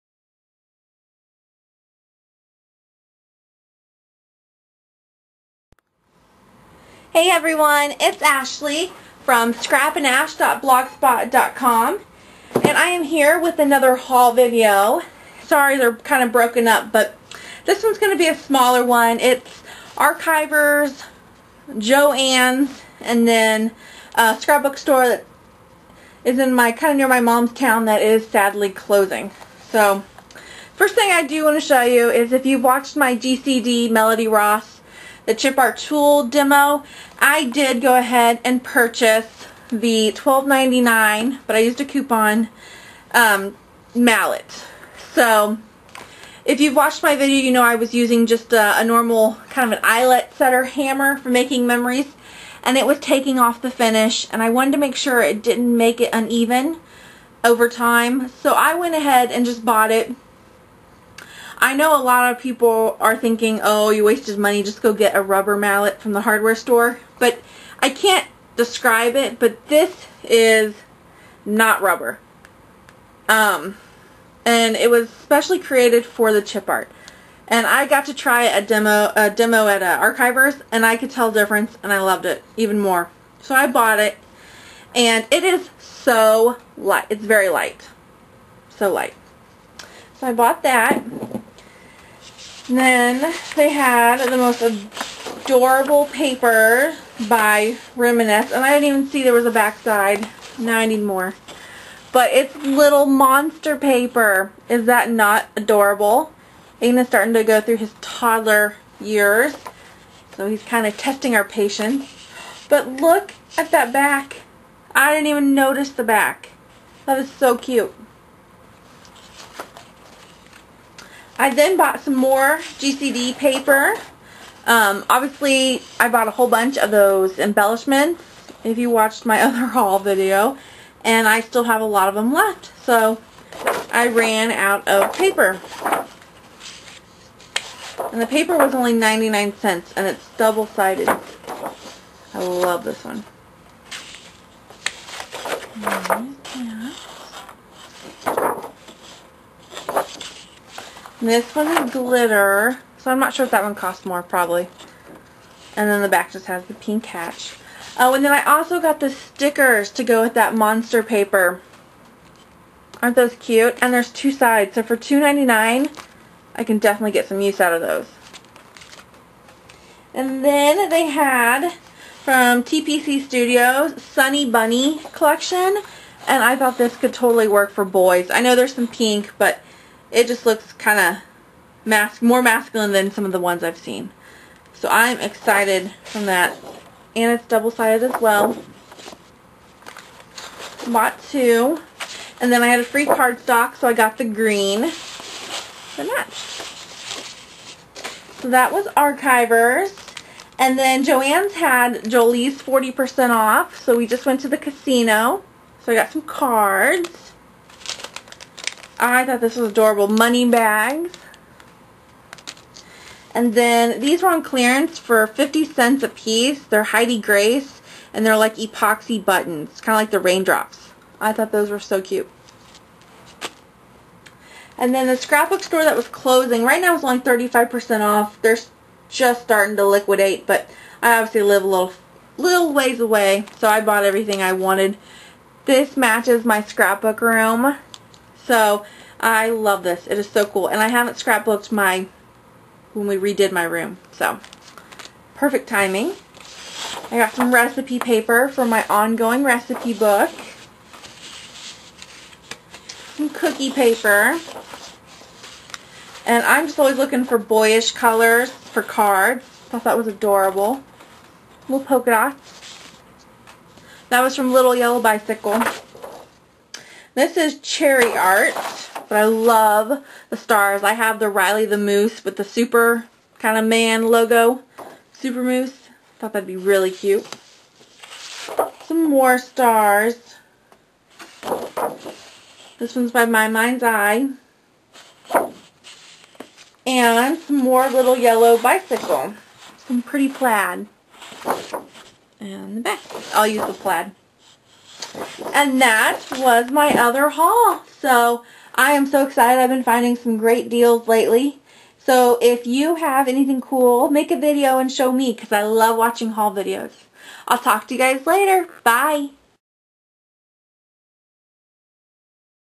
Mini Haul (Archivers, Jo Anns, LSS)
Hey Everyone,

Here I am with a mini haul from several shops and I wanted to share my goodies with you